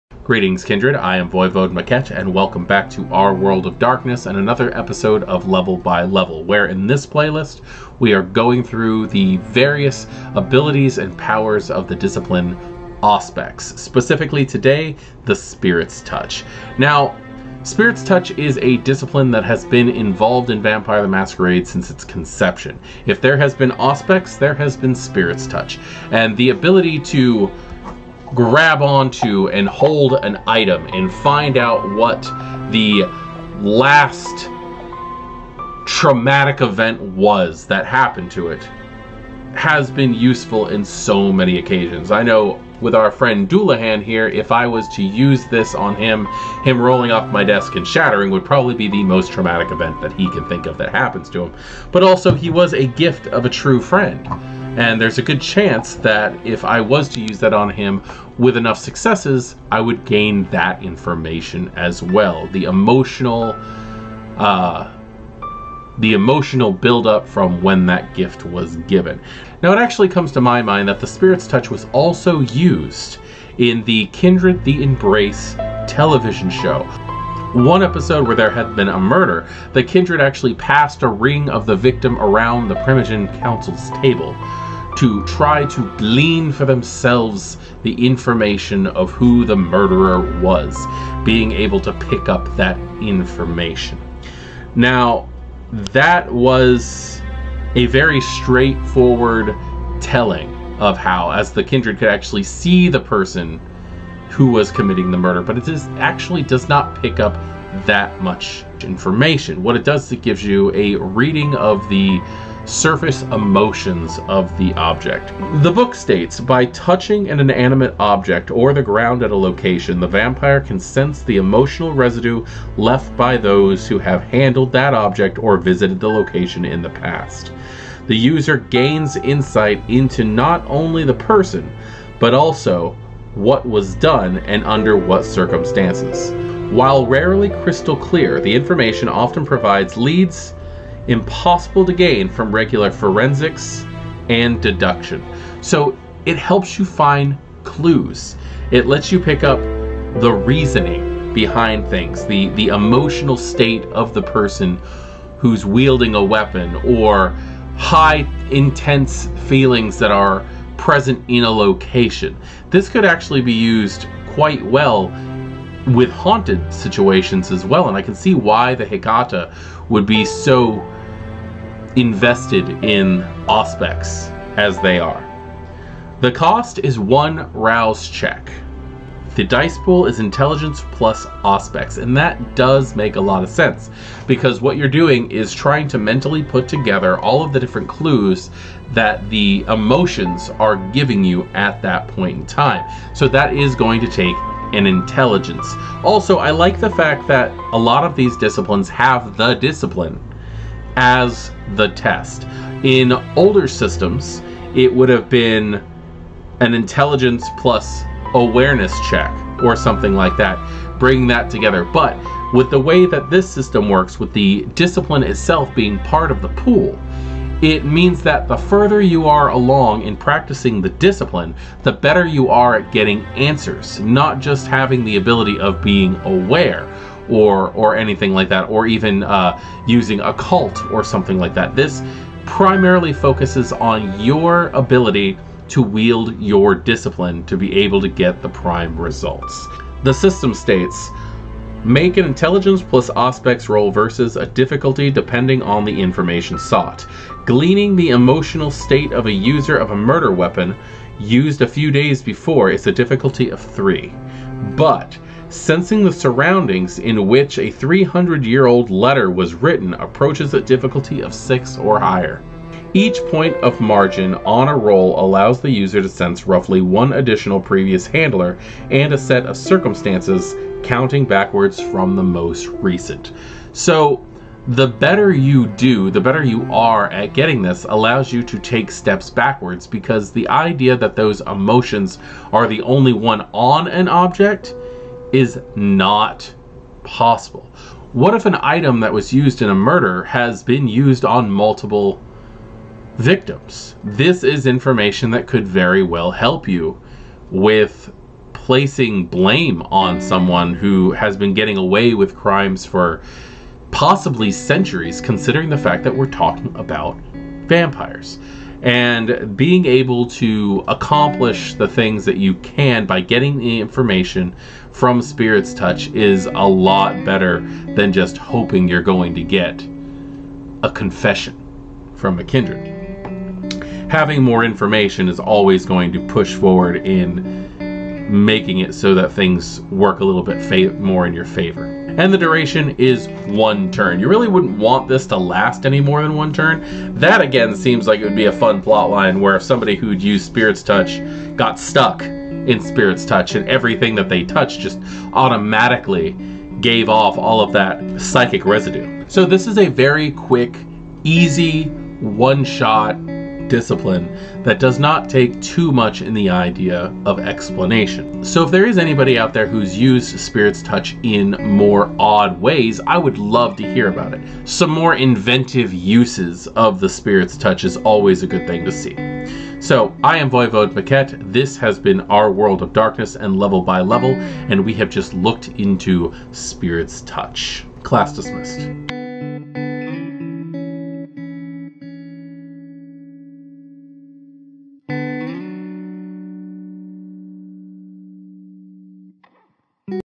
Level By Level: Auspex "Spirits touch"
Greetings Kindred, welcome to level by level, Auspex 101. Tonight we are looking into the power of Spirits Touch and how it has changed though out the history of VtM.